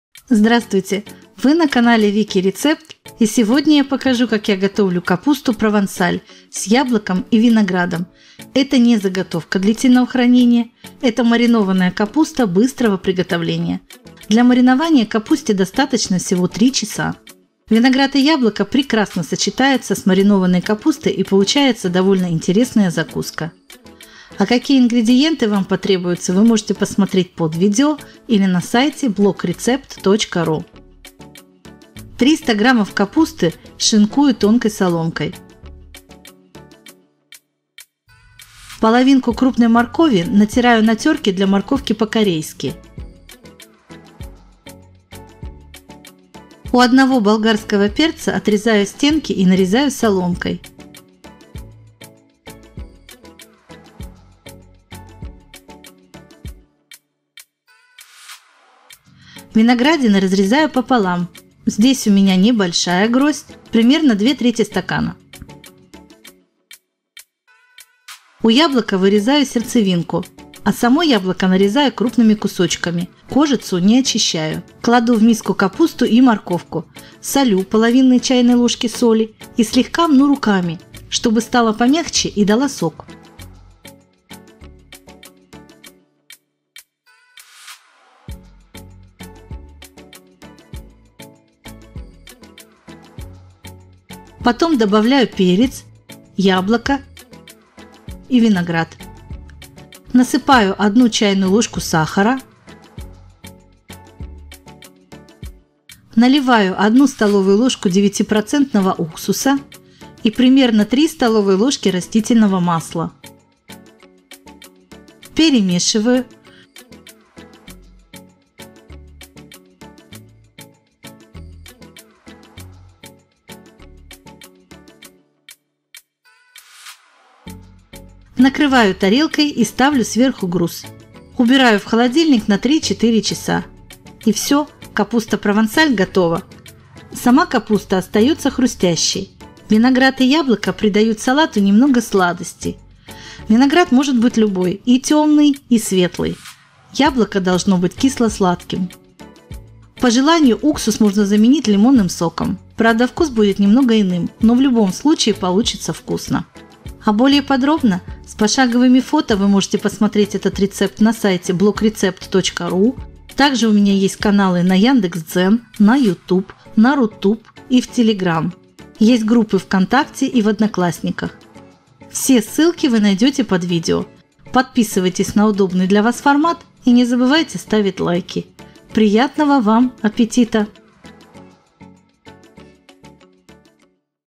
КАПУСТА «Провансаль» с ВИНОГРАДОМ и ЯБЛОКОМ быстрого приготовления
Здравствуйте! Вы на канале Wiki Recept и сегодня я покажу, как я готовлю капусту провансаль с яблоком и виноградом. Это не заготовка длительного хранения, это маринованная капуста быстрого приготовления. Для маринования капусте достаточно всего 3 часов. Виноград и яблоко прекрасно сочетаются с маринованной капустой, и получается довольно интересная закуска.

• виноград – 1 небольшая гроздь;
Количество: 5-6 порций

Смотреть этот рецепт на сайте blog-recept.ru с пошаговыми фото и текстом